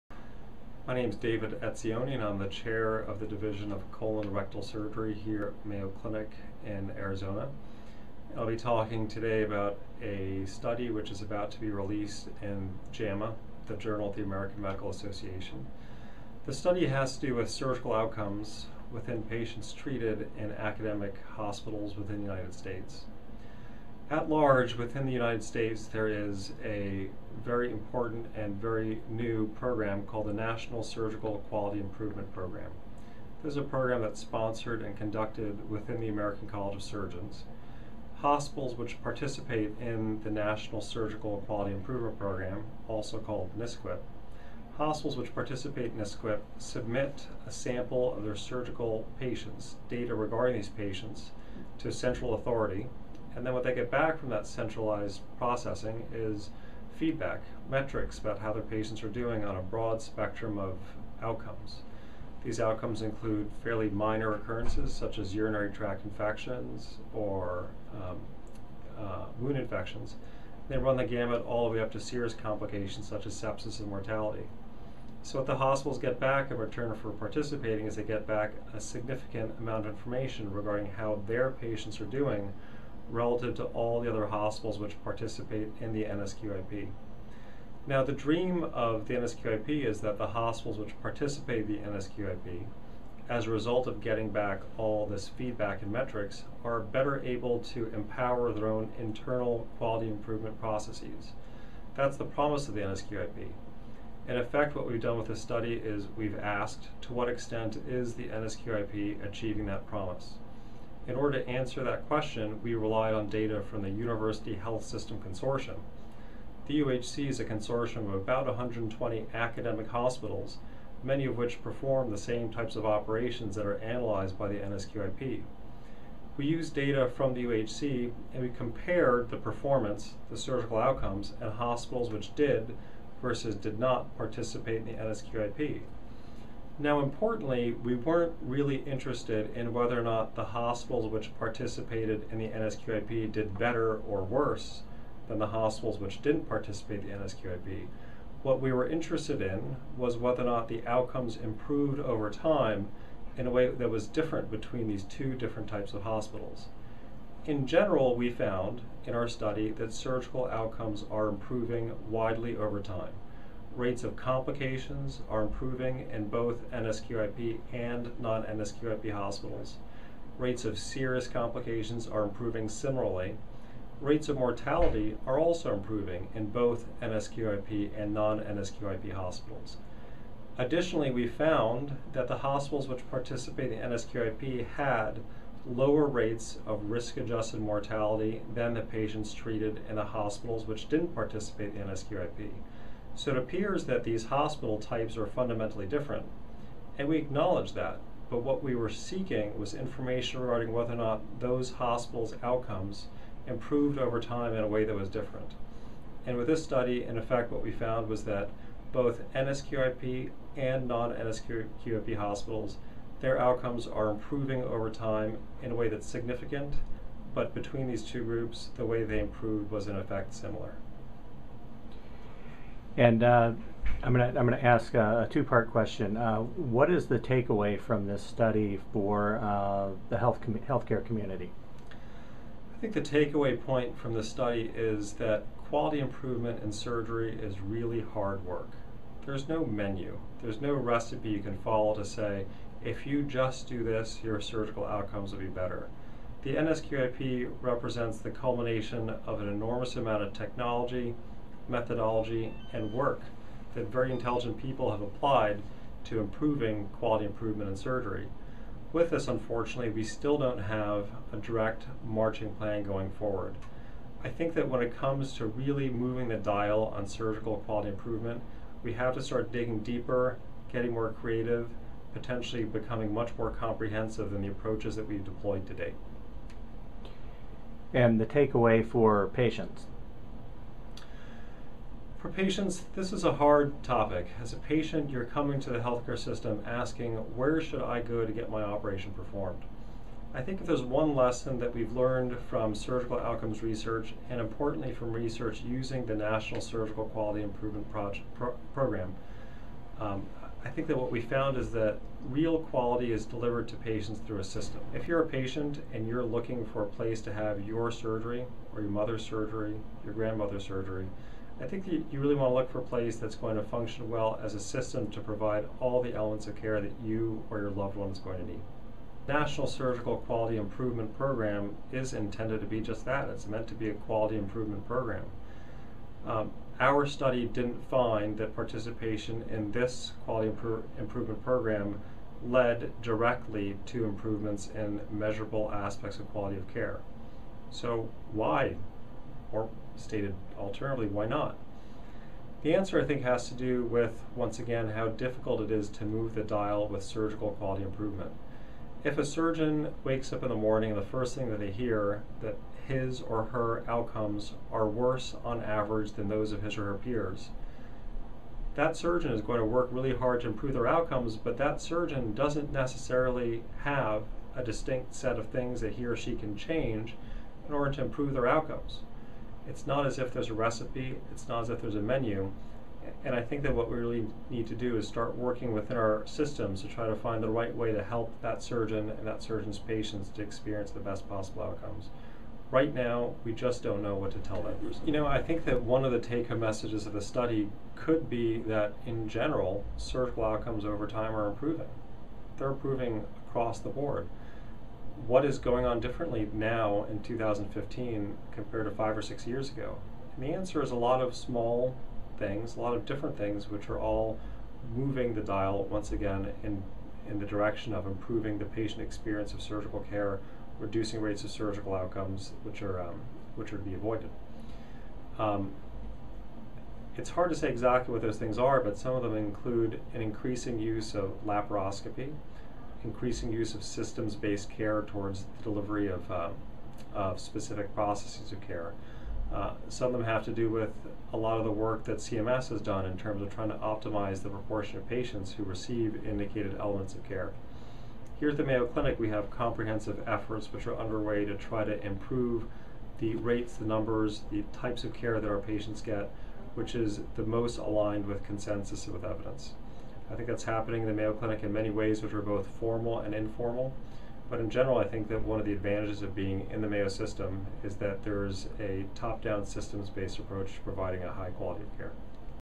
Mayo Clinic: Surgical Metrics Do Not Provide a Clear Path to Improvement
While surgical outcomes have improved nationally over time, surgical outcome reporting does not necessarily lead to better outcomes, according to a Mayo Clinic study published this week in the Journal of the American Medical Association.
Systems that capture, analyze, and report surgical outcomes are an increasingly important part of the quality improvement movement in health care in the United States.  Within the U.S., the most widely used surgical outcomes reporting system is the National Surgical Quality Improvement Program, which is coordinated through the American College of Surgeons. Study author, Dr. David Etzioni comments